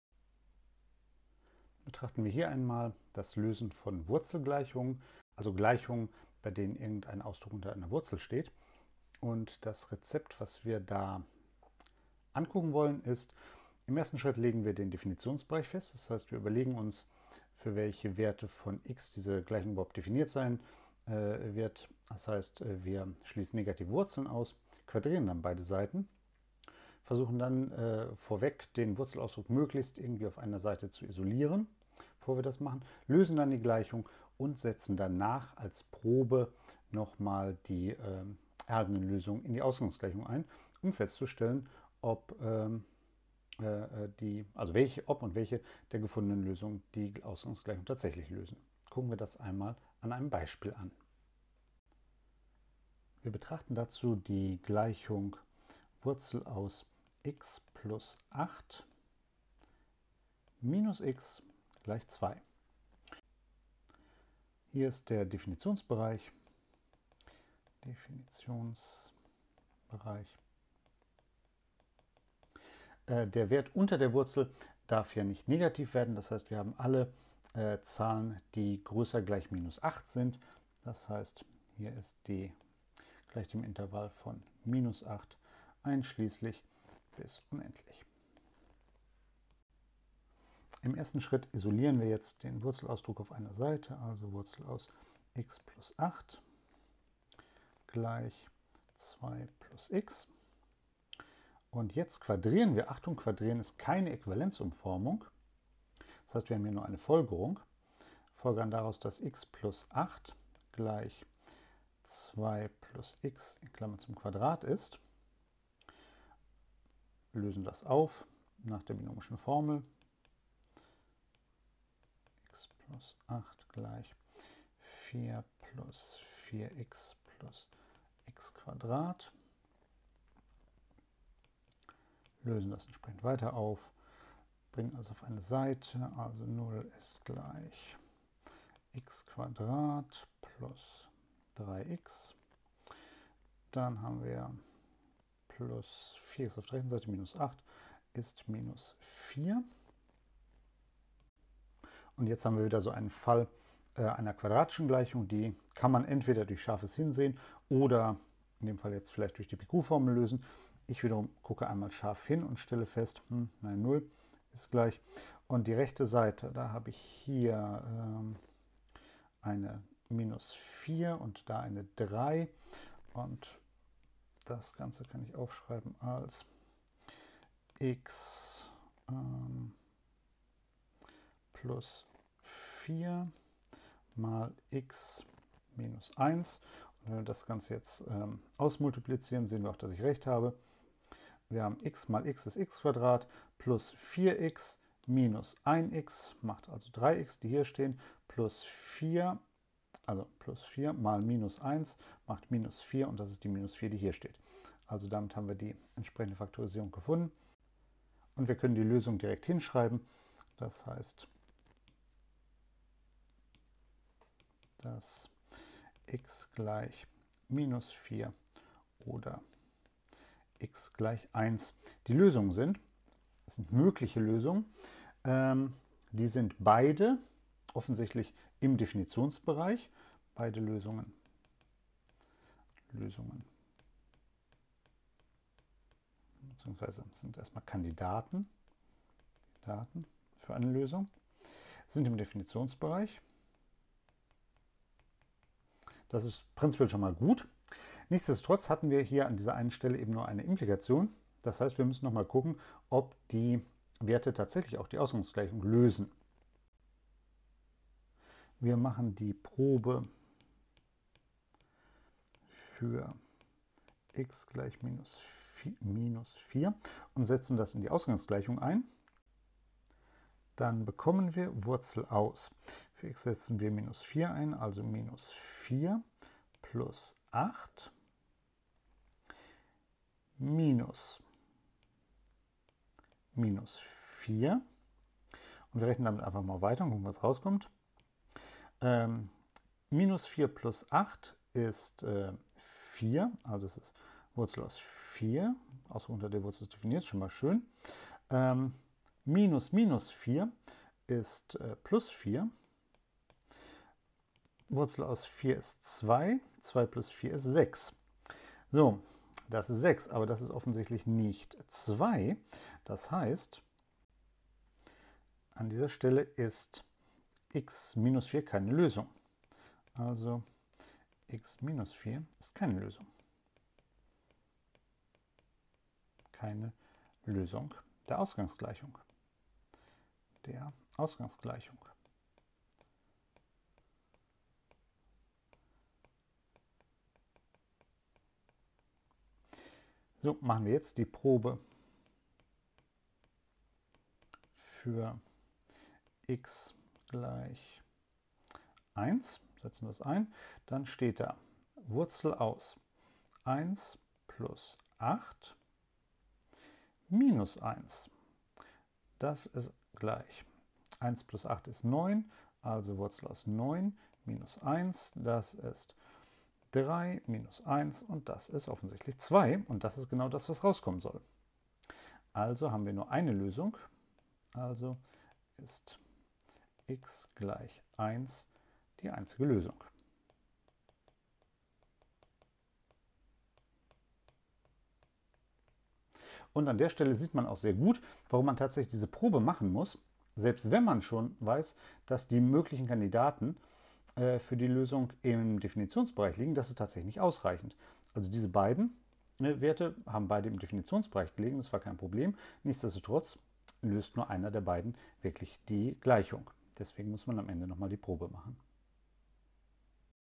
Gleichungen und Ungleichungen - Wurzelgleichungen
Wir illustrieren das Lösen von Wurzelgleichungen anhand eine Beispiels. Das Beispiel zeigt auch, dass Quadrieren keine Äquivalenzumformung ist.